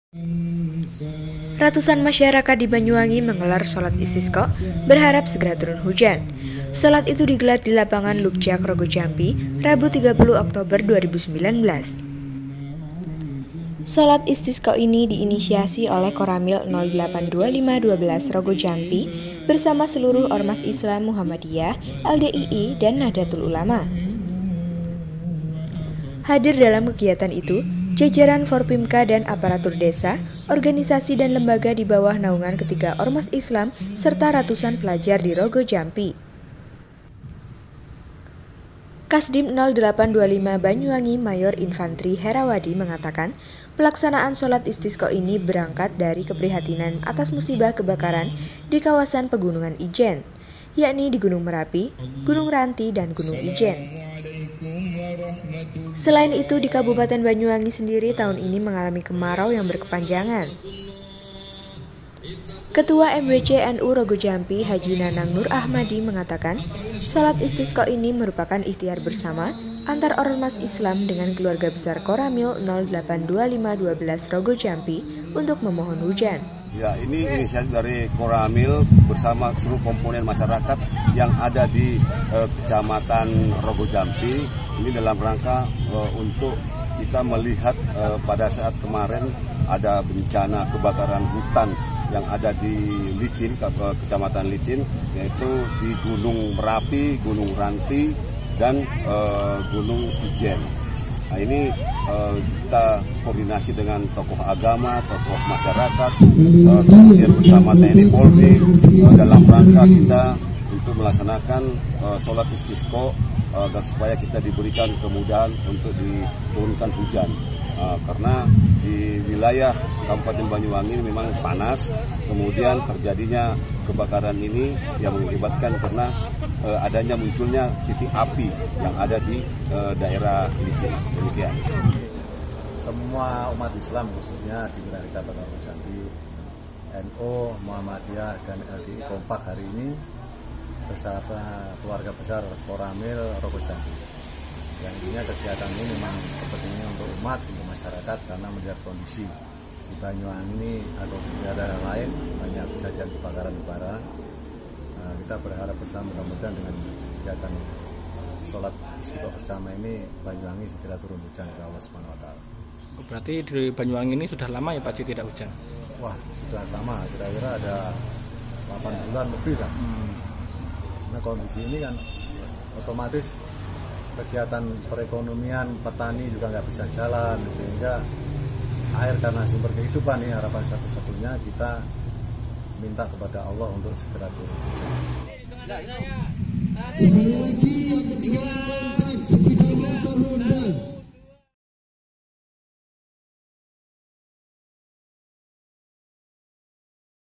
Mengharap Hujan, Warga Banyuwangi Laksanakan Salat Istisqa
Ratusan masyarakat di Banyuwangi menggelar Salat Istisqo berharap segera turun hujan. Salat itu digelar di Lapangan Lugjag Rogojampi, Rabu (30/10/2019).

Salat Istisqo ini diinisiasi oleh Koramil 0825/12 Rogojampi bersama seluruh ormas Islam, Muhammadiyah, LDII, dan Nahdlatul Ulama.

Reporter: Hafilludin Ahmad
Pengisi Suara: Marta Junisa
Editor Video: Fajar Mujianto